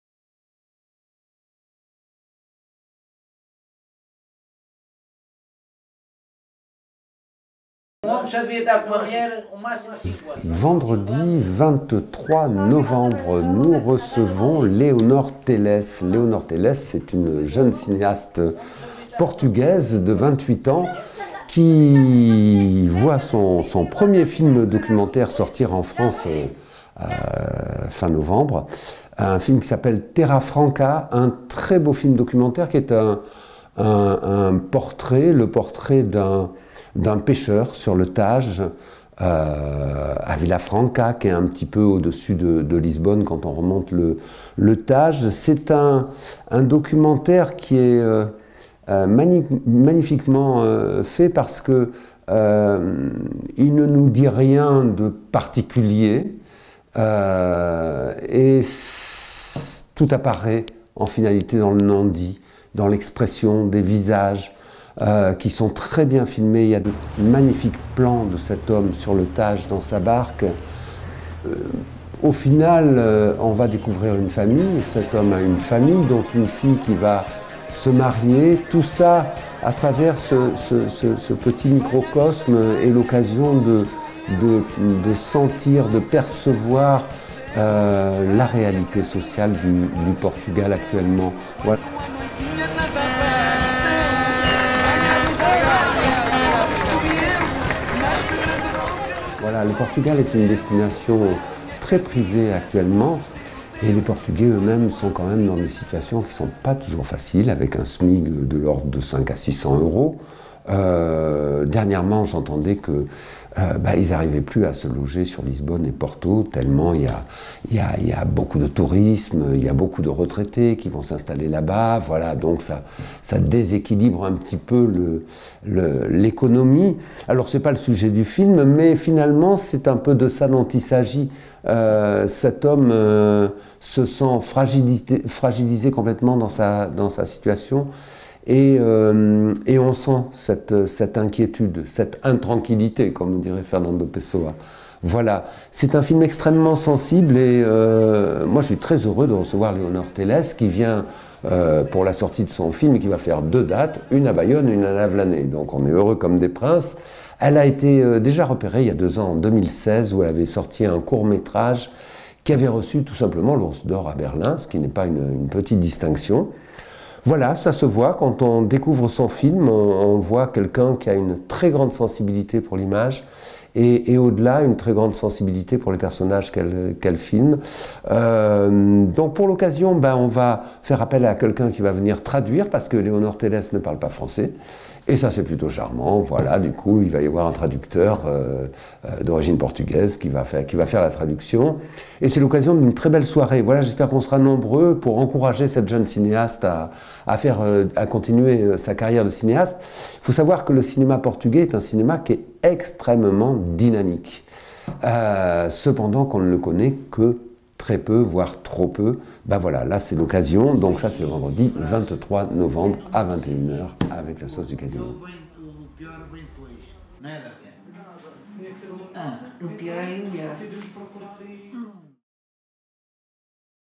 Une semaine - Un film : "Terra Franca" de Leonor Teles
Cette semaine, la Sauce du Casino propose un film documentaire portugais de la jeune réalisatrice Léonor Tores, Terra Franca